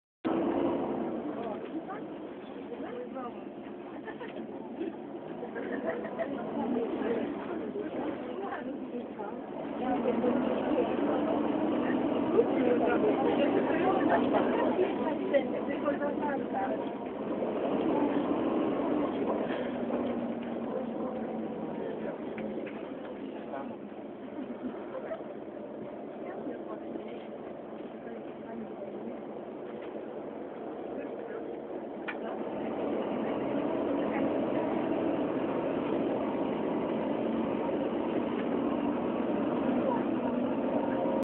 Legionowo - Zagubione 723
Kierowca Mana pomylił trasę. Nie skręcił z Warszawskiej w lewo w Sobieskiego tylko pojechał prosto i zawraca przez stację Shell.